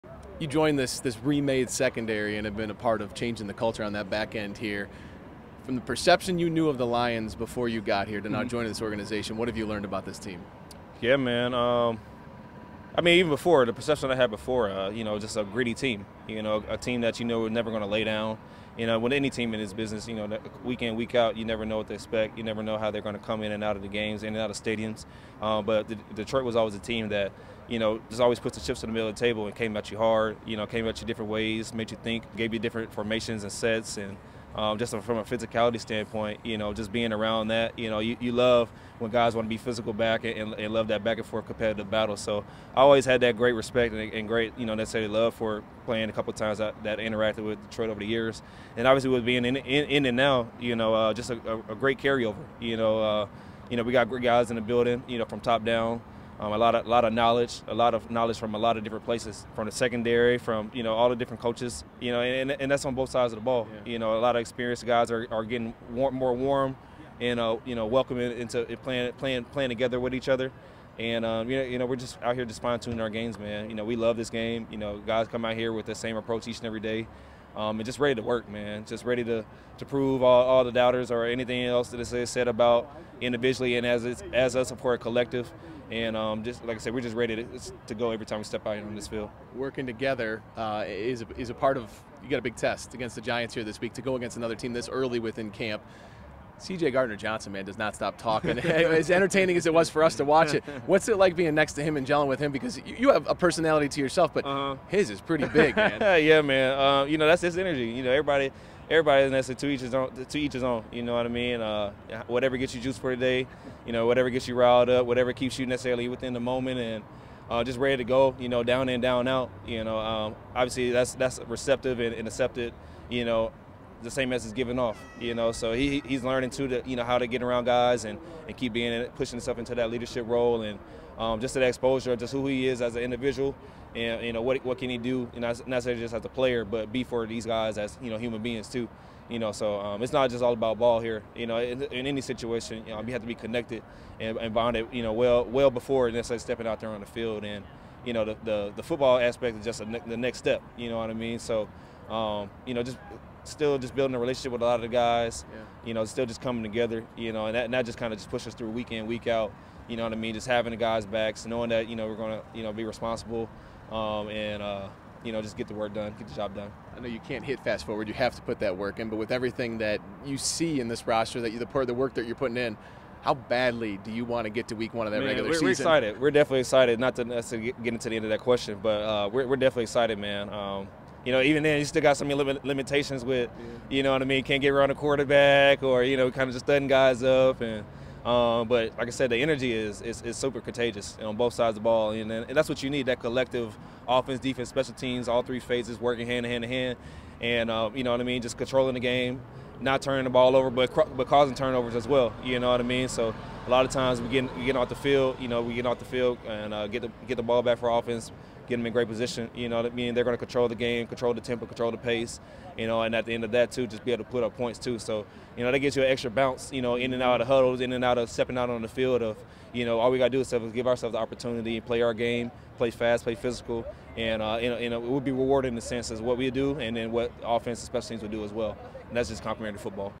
INTERVIEW Cam Sutton talks bolstered Lions secondary, hunger to start season
INTERVIEW: Cam Sutton signed with the Lions to make over the secondary.

We talked about his perception of the team before and after signing, bonding on the field with CJ Gardner-Johnson, and the excitement to start the season.